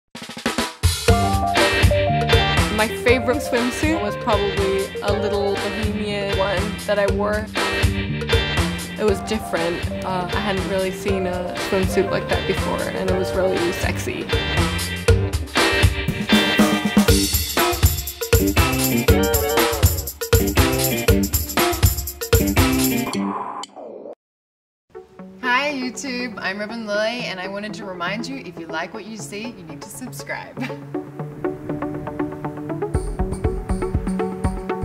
Kelly Gale Wears Nothing But A Necklace & Bikini Bottoms | Sports Illustrated Swimsuit
Kelly Gale, SI Swimsuit 2017 rookie, takes you to her Sumba Island paradise in this revealing photoshoot.